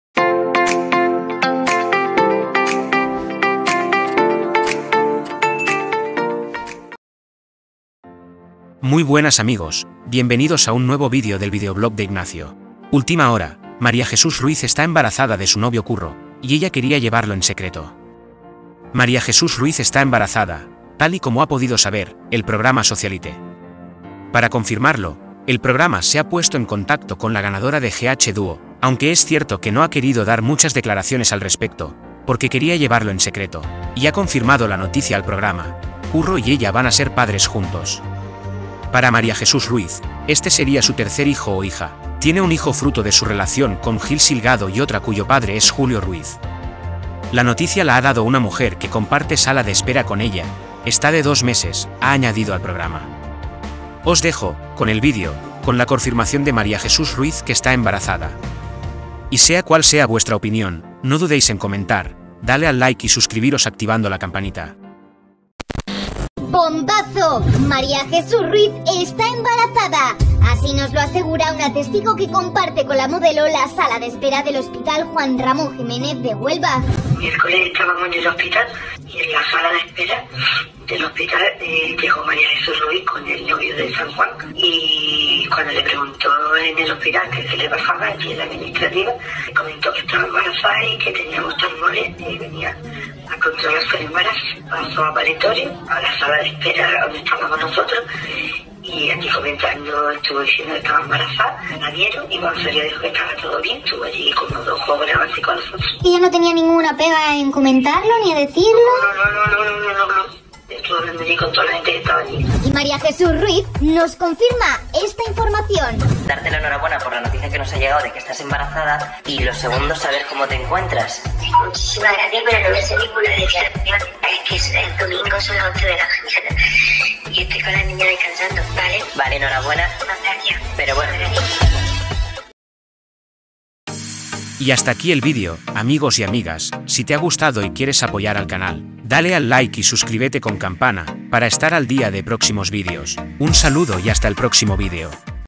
🔴 MARIA JESUS RUIZ está EMBARAZADA de su novio CURRO quería LLEVARLO en SECRETO
María Jesús Ruiz está embarazada, tal y como ha podido saber, en exclusiva 'Socialité'. Para confirmarlo, el programa se ha puesto en contacto con la ganadora de 'GH Dúo' y, aunque es cierto que no ha querido dar muchas declaraciones al respecto (porque quería llevarlo en secreto), nos ha confirmado la noticia: Curro y ella van a ser padres juntos.

María Jesús Ruiz confirma que está embarazada
Para María Jesús Ruiz, este sería su tercer hijo o hija (tiene una fruto de su relación con Gil Silgado y otra cuyo padre es Julio Ruz). La noticia nos la da una mujer que comparte sale de espera con ella: "Está de dos meses", añade. Nos ponemos en contacto con María Jesús para darle la enhorabuena y agradece nuestra felicitación.